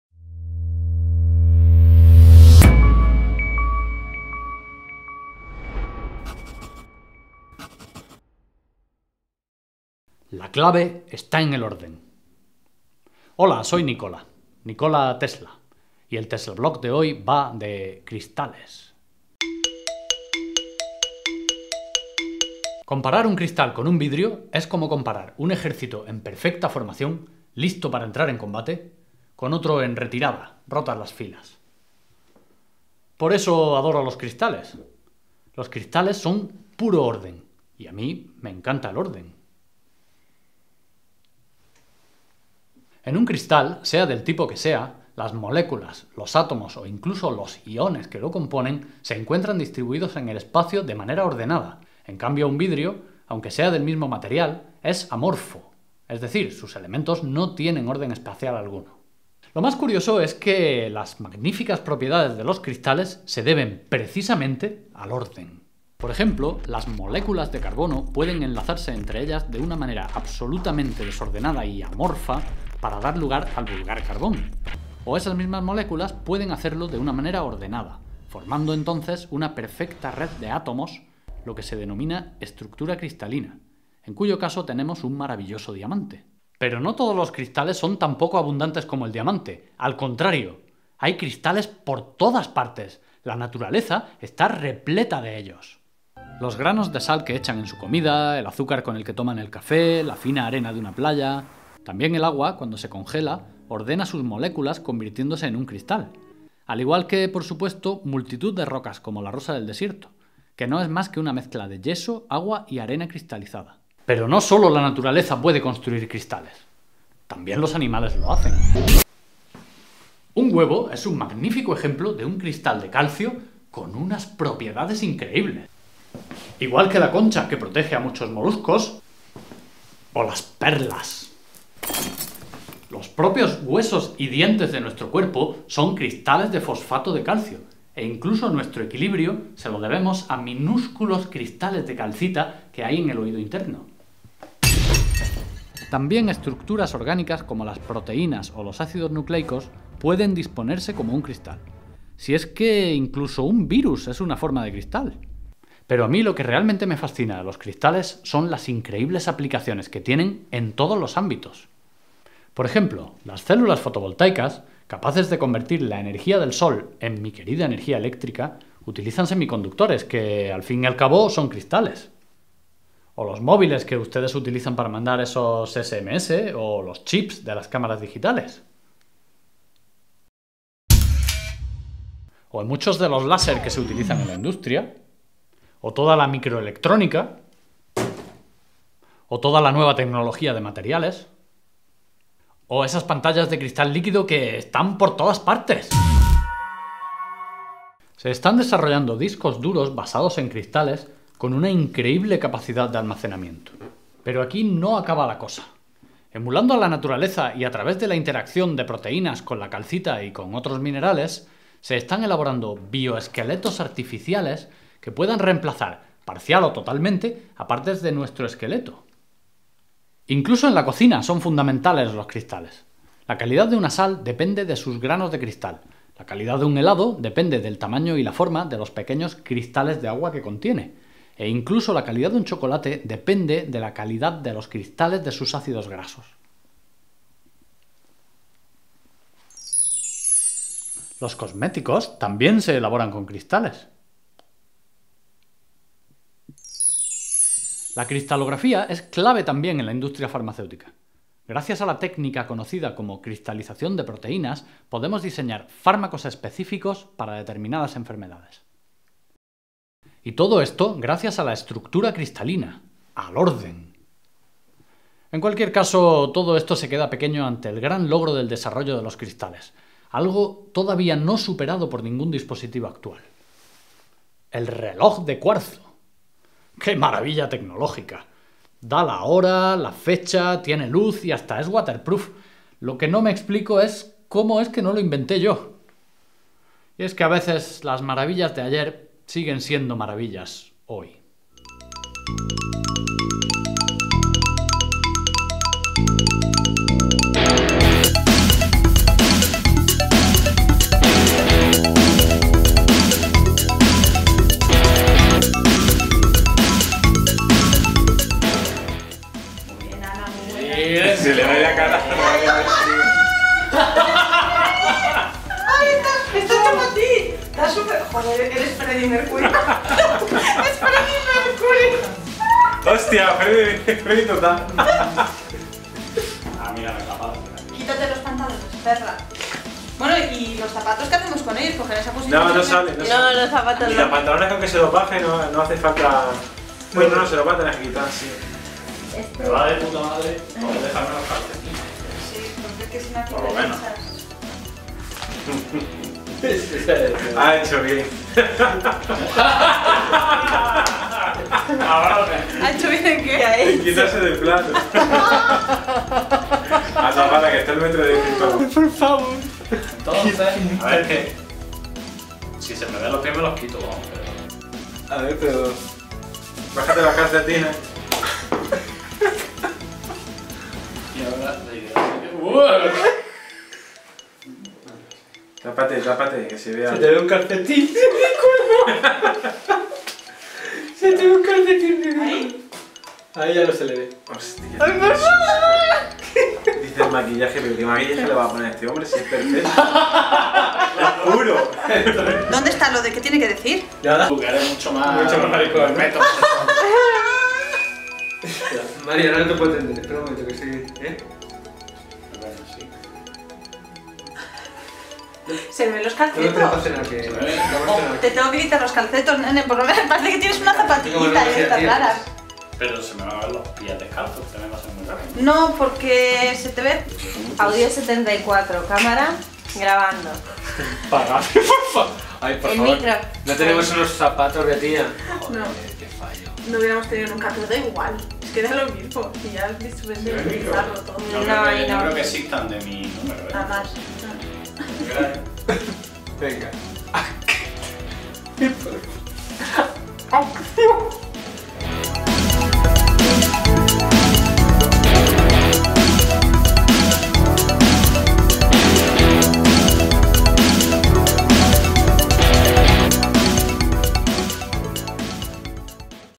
Teslablog #21 - Tesla y los cristales
A pesar de no mencionarlos en ninguno de los textos que publicó en vida, no era de extrañar que Tesla terminara dedicando un teslablog completo a los cristales. Como él mismo indica: “Los cristales son puro orden, y yo adoro el orden”. Es cierto que las propiedades de estos fabulosos materiales, conocidos desde la antigüedad, no se pudieron relacionar con la disposición espacial de sus átomos o iones –la llamada estructura cristalina– hasta la llegada de la difracción de rayos X, pero también es cierto que se trata de una técnica que nuestro inventor podría haber acabado desarrollando de contar con tiempo vital suficiente (Tesla fue uno de los primeros en experimentar con la radiación X, incluso antes que Röntgen).